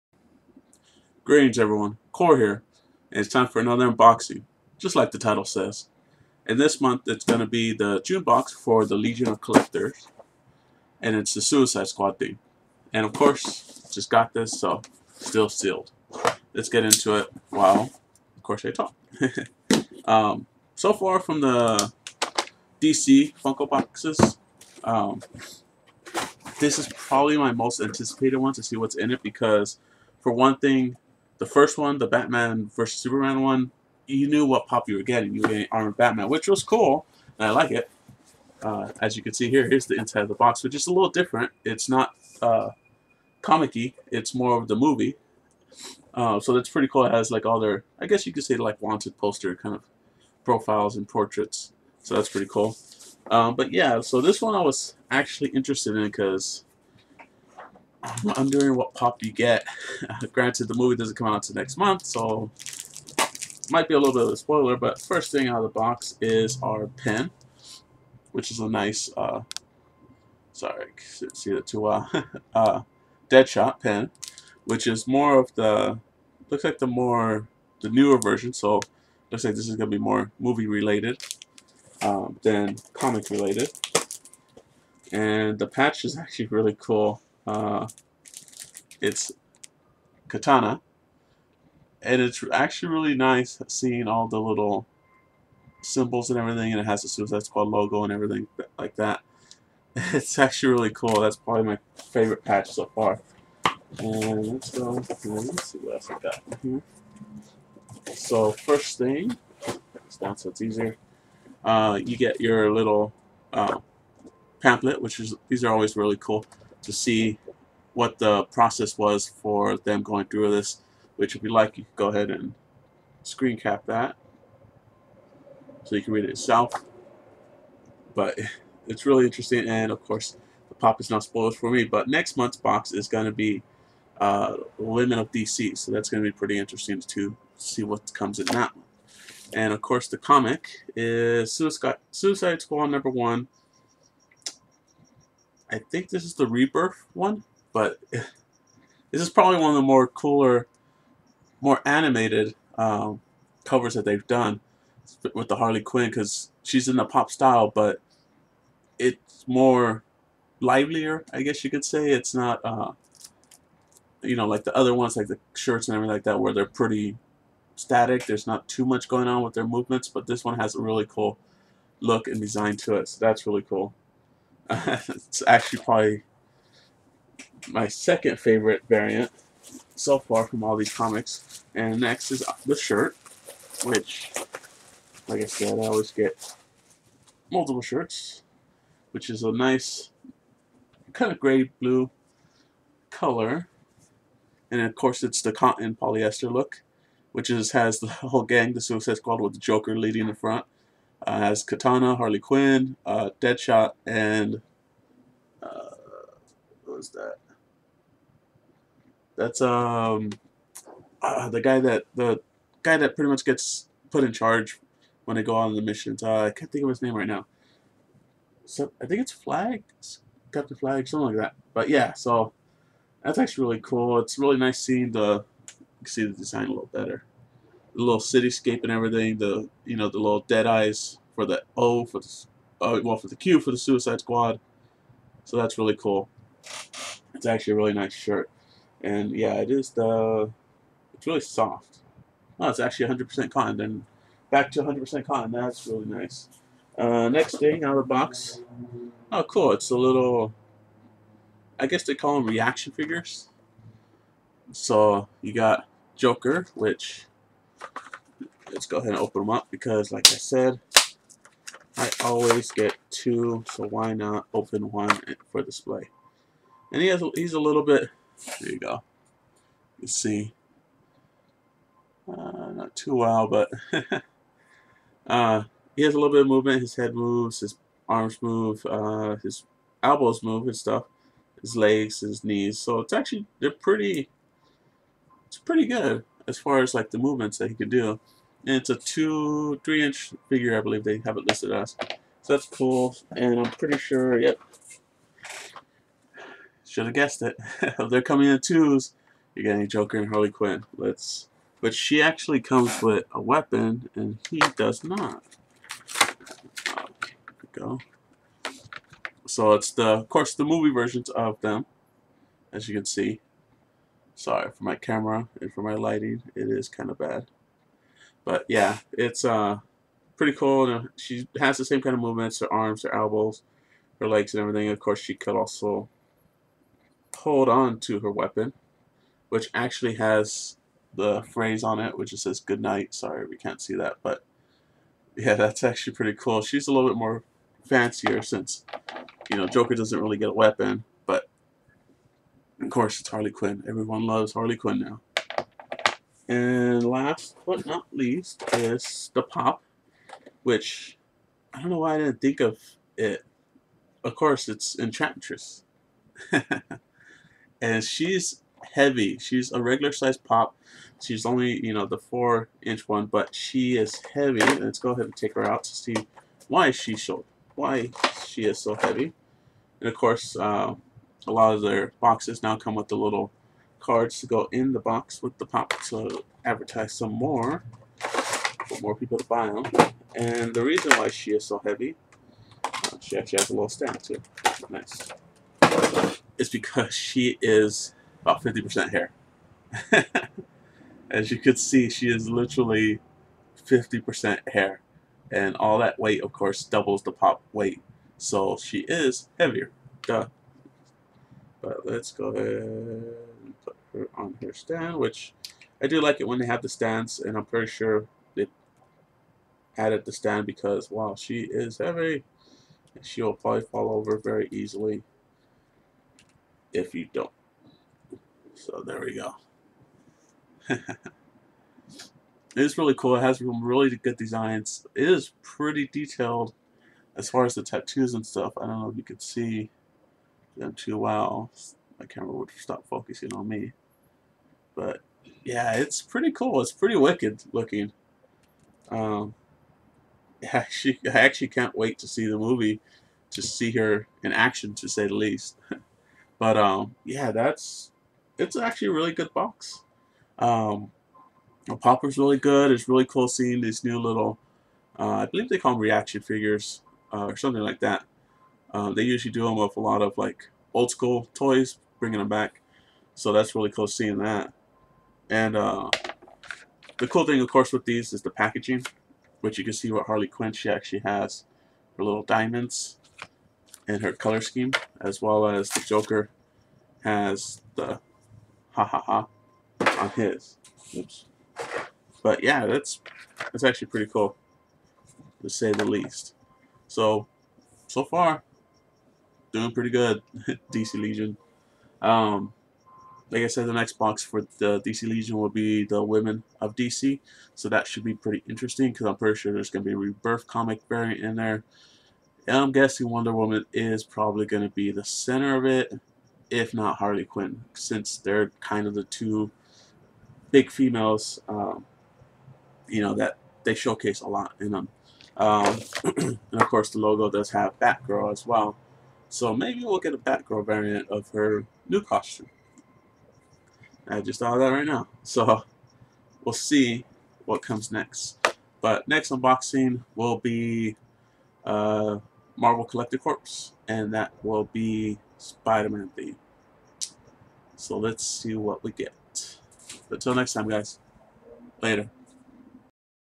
Unboxing Funko's DC Legion of Collectors - Suicide Squad box (July 2016)
The Squad's assembled and ready to help the world, even though they tried to destroy it before, let's see what's inside.